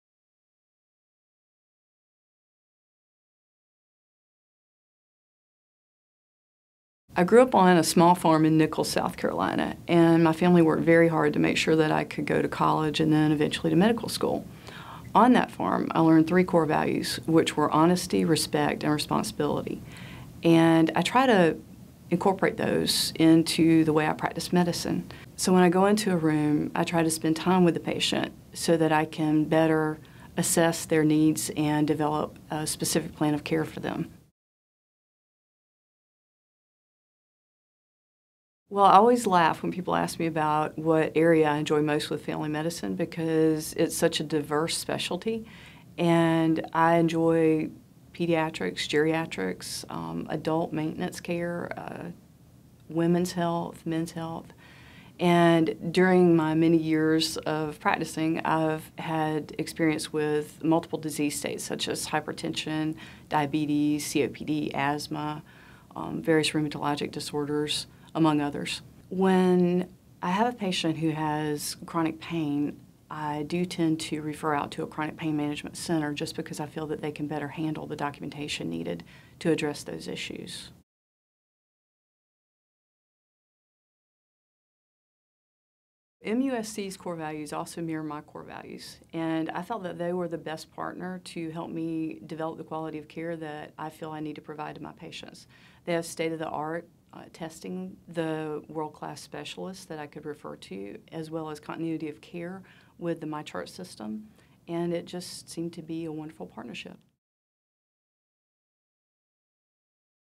Dr. Kim Jackson, Primary Care - Family Medicine - MUSC Health
Kimberly Jackson, M.D., is board certified by the American Board of Family Medicine and provides primary care for men, women, and children.  Dr. Jackson grew up on a small farm in Nichols, SC. After earning her Bachelor of Science degree from Coastal Carolina University, she attended MUSC in Charleston, where she graduated with a Doctorate in Medicine. From there, she completed her Family Medicine Residency training in Florence, SC. In the years that followed, she owned her own family medicine practice in Conway, SC where she established a reputation for being thorough and compassionate.

She and her husband currently reside on a horse farm in the low country. They have established a ministry called God's Horses through which they reach out to various non-profit organizations such as the Connie Maxwell Children's Home to offer educational visits to the farm as well as the benefit of the calming effects of interaction with the horses.
Dr. Jackson's professional interests include every aspect of primary care from pediatrics to geriatrics, men and women's health issues, preventative medicine, chronic disease treatment { to include among others; hypertension, diabetes, cardiovascular disorders, COPD, asthma, post stroke management, skin disorders, thyroid disorders) , minor surgeries such as skin biopsies and cryosurgery , and acute illness management as well as various psychiatric disorders. She feels that a person's physical well-being is closely connected to their emotional and spiritual well-being and makes every effort to truly "get to know" her patients in order to help them achieve their most healthful life.